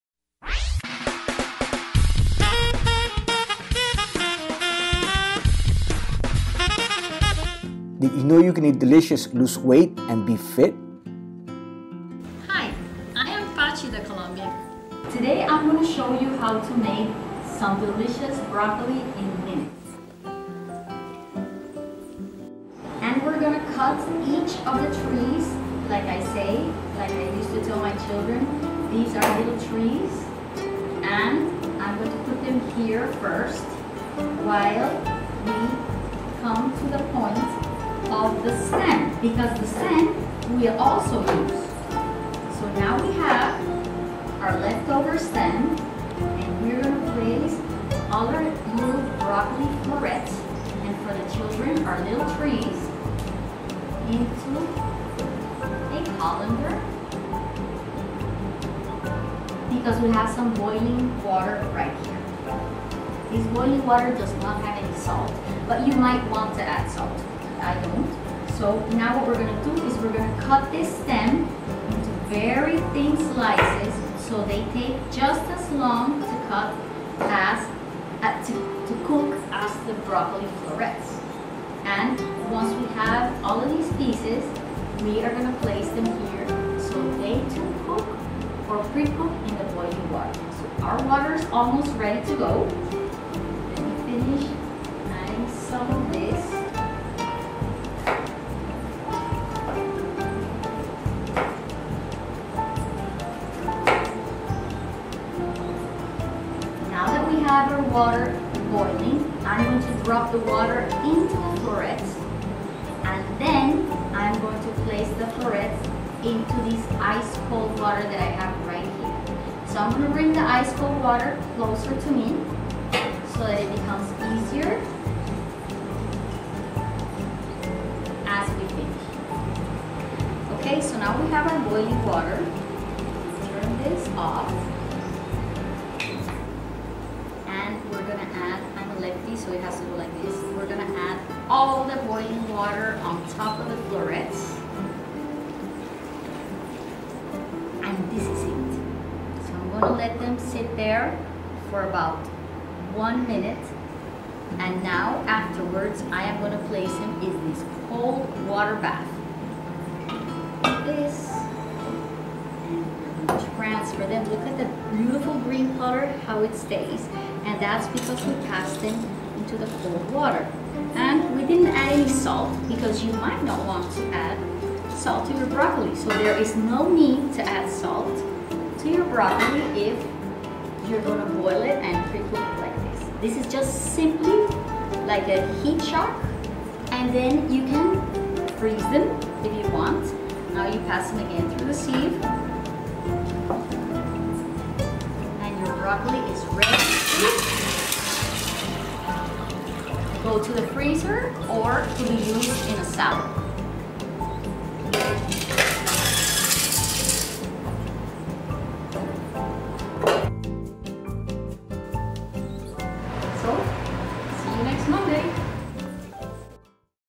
How to easy PRECOOK Fresh Broccoli in 1 minute
Delicious Diet are Sustainable Diet, with Chef Pachi!
Welcome to Pachi's The Colombian Cook, Today I want to show you the strong benefits of brocolli, being a cruciferous vegetable, and the different ways of integrating it with your meals.

by Pachi